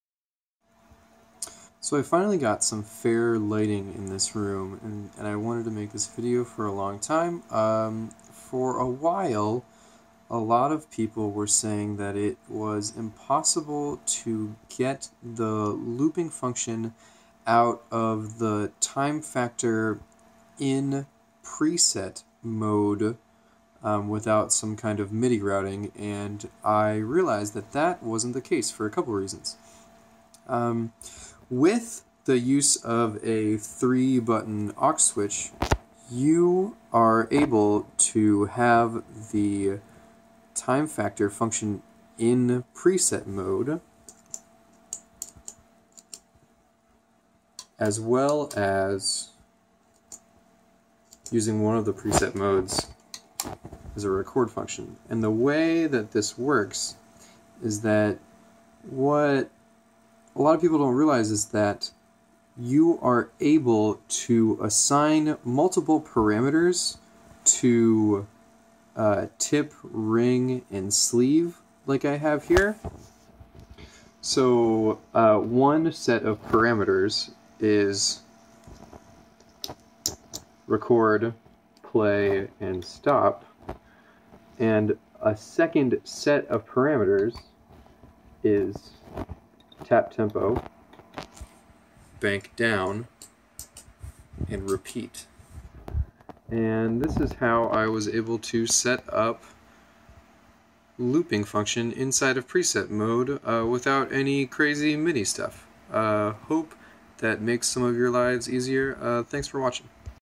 Timefactor Looping in Preset Mode using 3 button aux switch
I realized that because of the ability to assign multiple parameters to TRS in the Timefactor, it's possible to use a looper INSIDE OF preset mode, something that many people seemed to have difficulty doing. Hope this clarifies some things!

My Mapping
TIP: Tap Tempo, Stop Loop
RING: Bank Down, Play Loop
SLEEVE: RPT ON/OFF, Record Loop